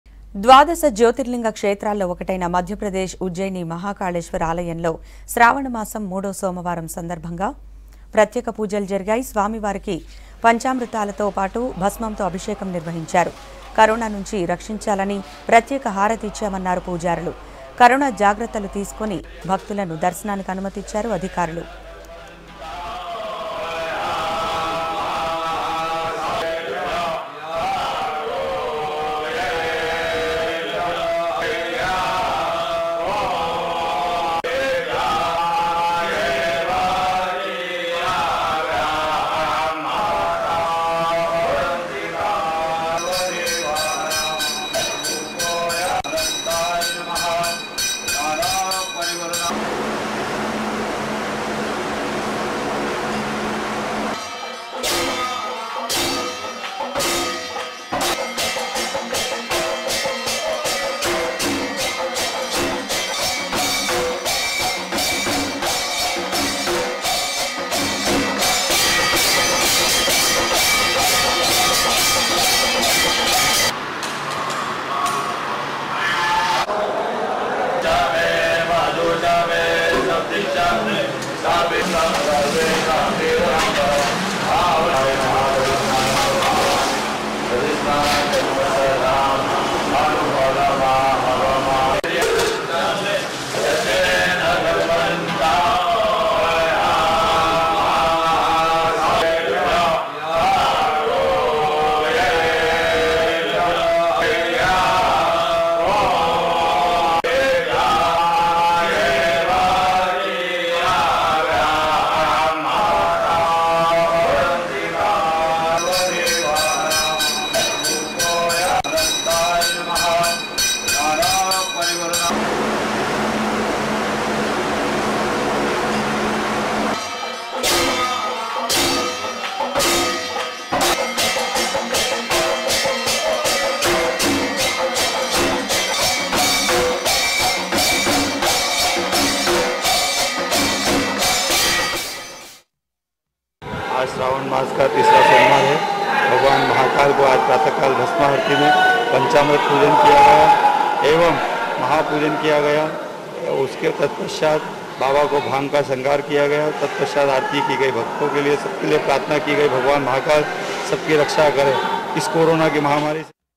Madhya Pradesh Ujjain Mahakaleshwar Temple Special Rituals On Occasion Of Sravana Masam | V6 News
Madhya Pradesh Ujjain Mahakaleshwar Temple Special Rituals On Occasion Of Sravana Masam